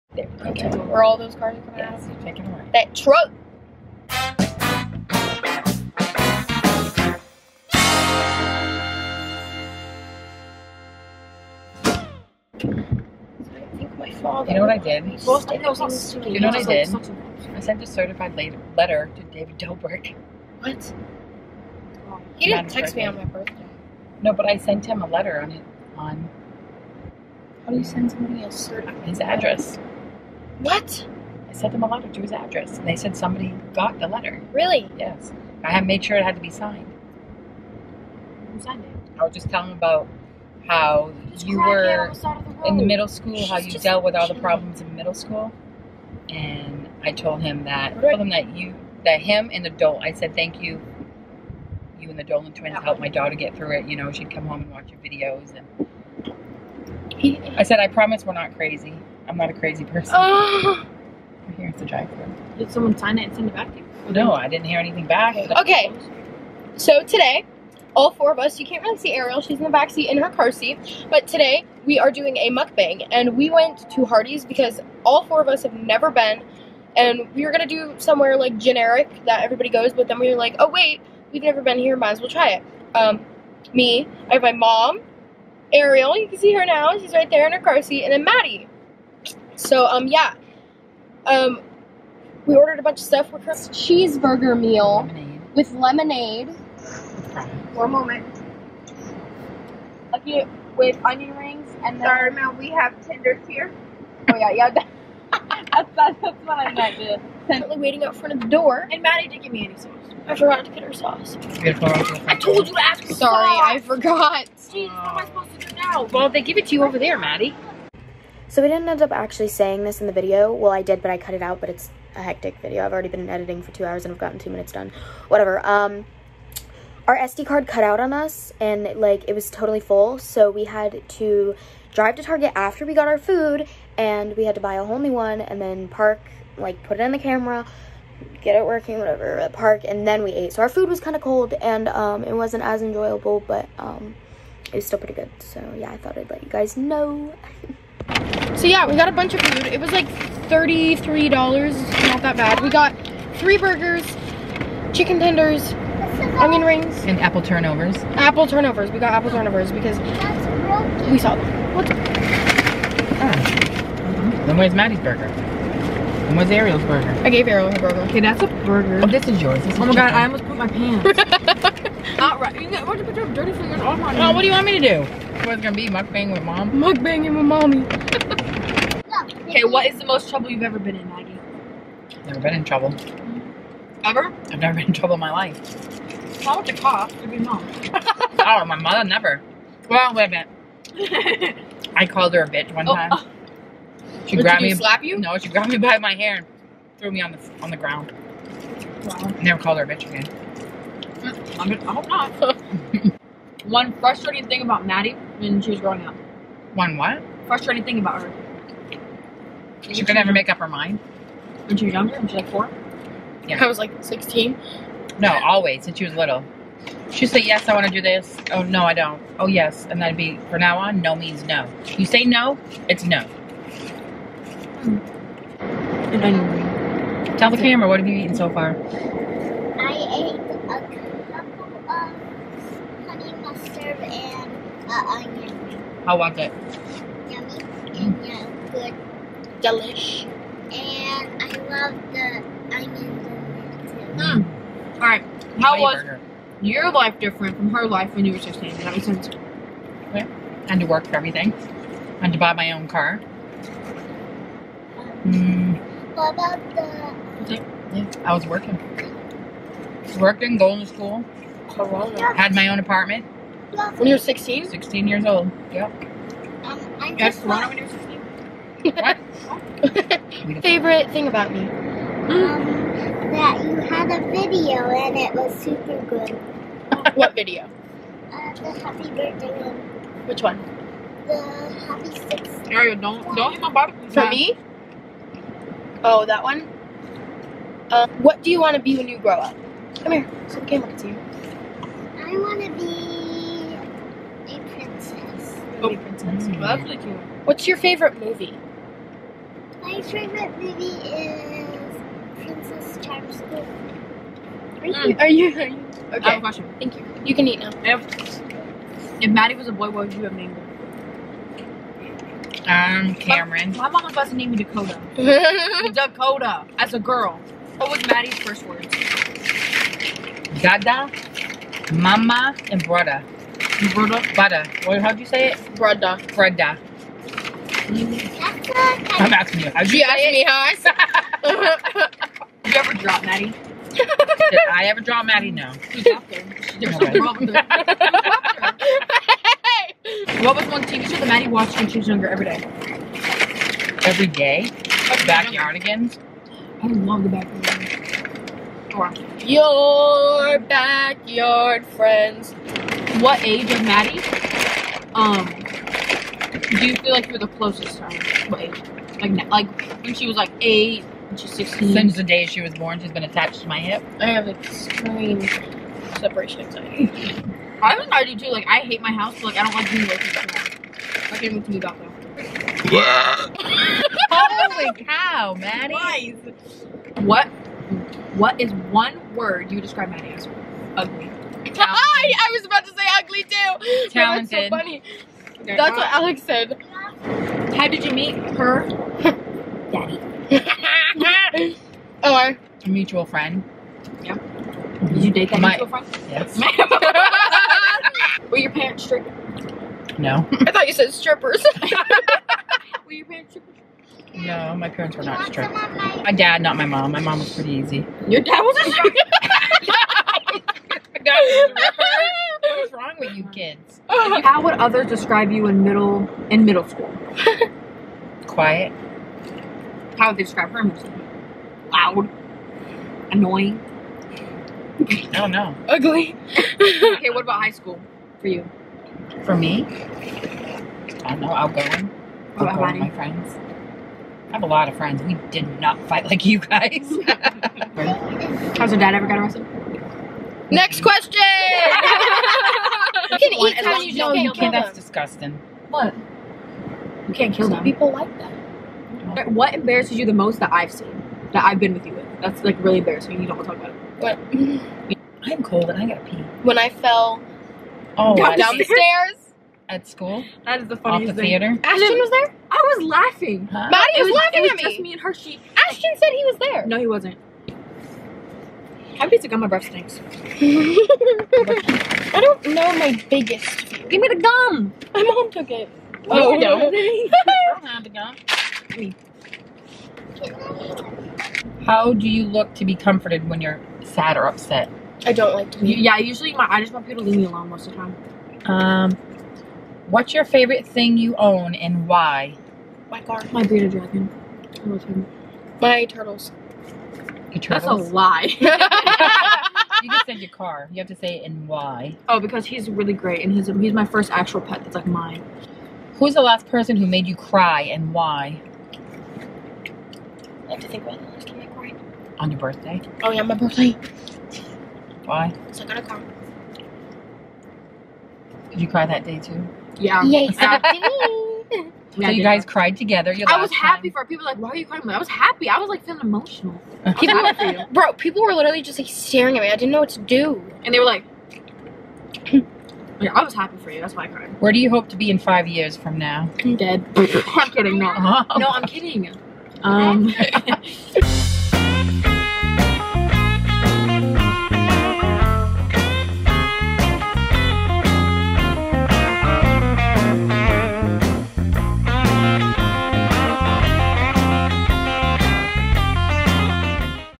Hardees Mukbang/Q&A ft. Mom and Best Friend
In this video My Mom and My Best friend came along with me to film a Mukbang at Hardees we have never been so we thought it would be a great place to go. While we ate our food we did a short little Q&A.